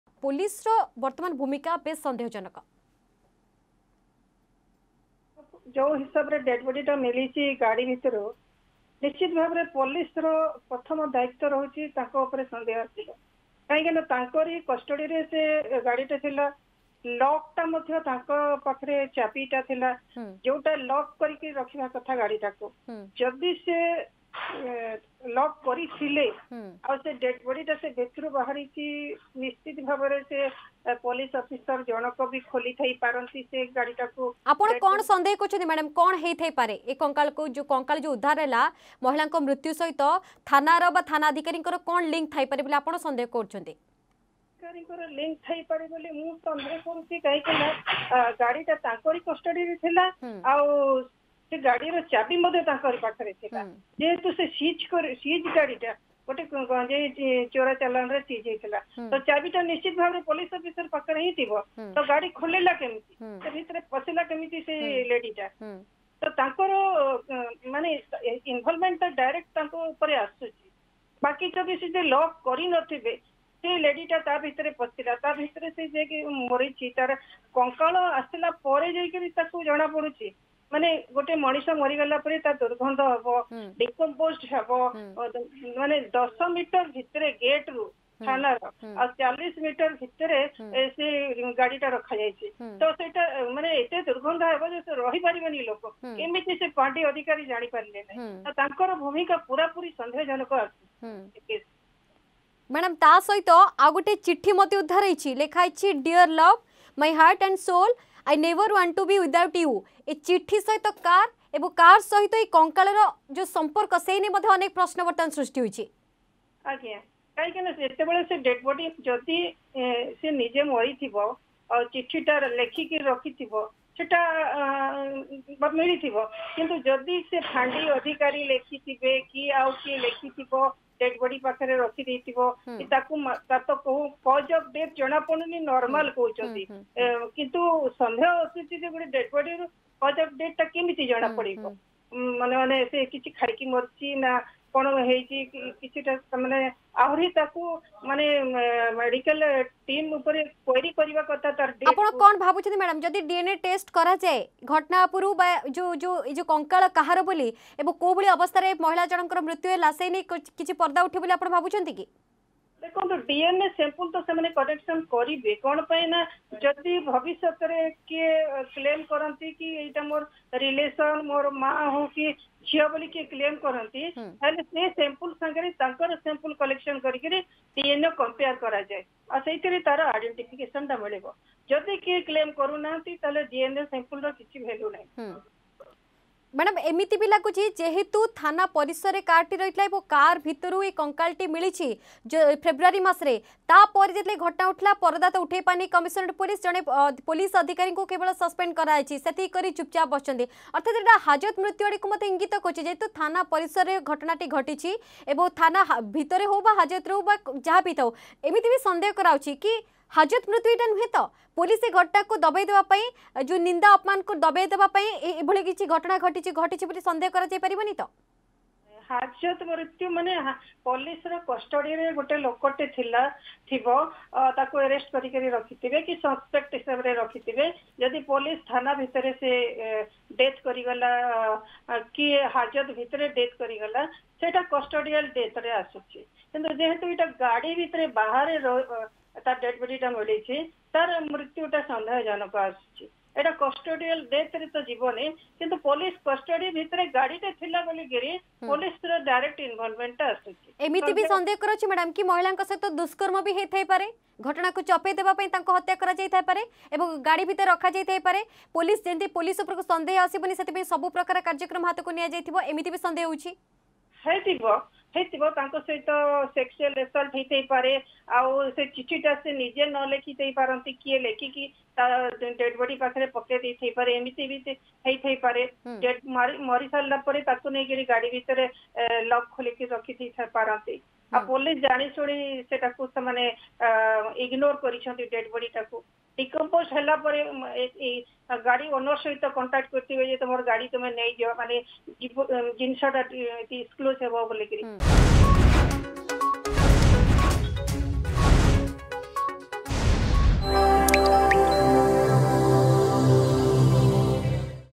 ପୋଲିସର କାର୍ଯ୍ୟକଳାପ ଉପରେ ସନ୍ଦେହ
ଗାଡି ପୋଲିସ ପାଖରେ ଥିଲା ବେଳେ ତା ଭିତରୁ ନରକଙ୍କାଳ ବାହାରିବା ନିହାତି ଭାବେ ପୋଲିସ ଉପରେ ସନ୍ଦେହ ଆସିବା ସ୍ବଭାବିକ...